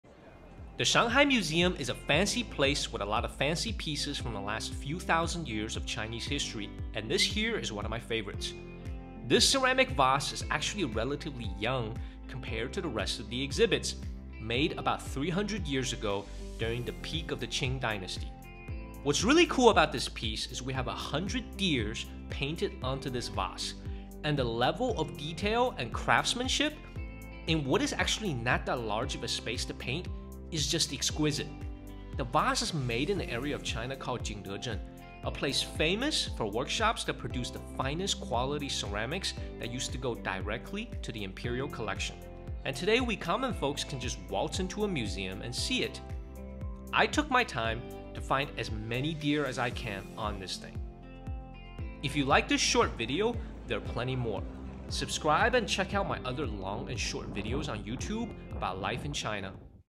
Ep.51 - Another masterpiece from the Shanghai Museum
This vase from the Qing Dynasty is called the 100 deer vase, made in Jingdezhen, a famous place for producing the finest quality ceramic wares that used to go directly into the emperor's collection!